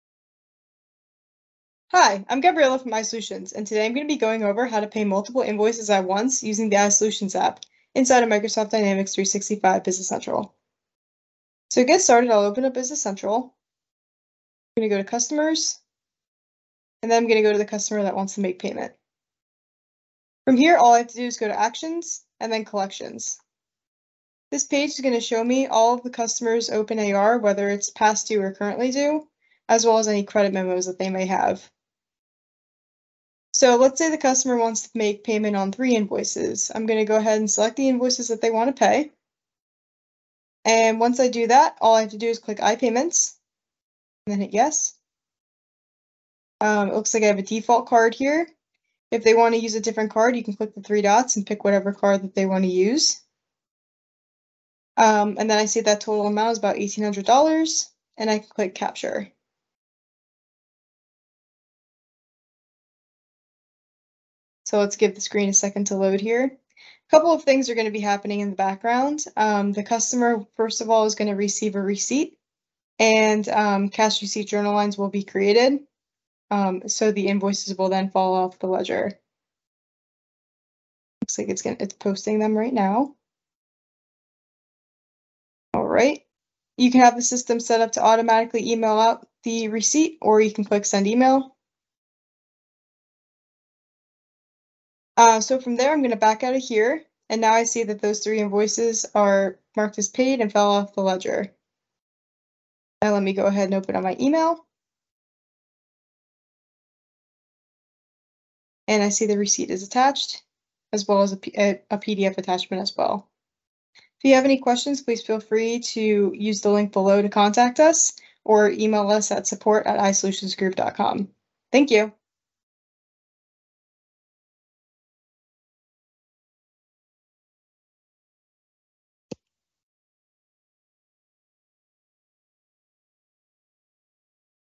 How to Pay Multiple Invoices at Once in Dynamics 365 Business Central | iSolutions App Tutorial
Looking for a faster way to pay multiple invoices in Microsoft Dynamics 365 Business Central? In this step-by-step tutorial, you'll see how to use the iSolutions Payments App to select and pay multiple open invoices in just a few clicks.

What You’ll Learn:

How to view all open AR and credit memos for a customer
How to select multiple invoices for payment
How to choose a saved or new credit card for payment
How to capture the total amount and process the transaction
How receipts are automatically generated and emailed
How paid invoices are removed from the ledger automatically

This feature helps you streamline your accounts receivable process, reduce manual work, and improve the customer payment experience.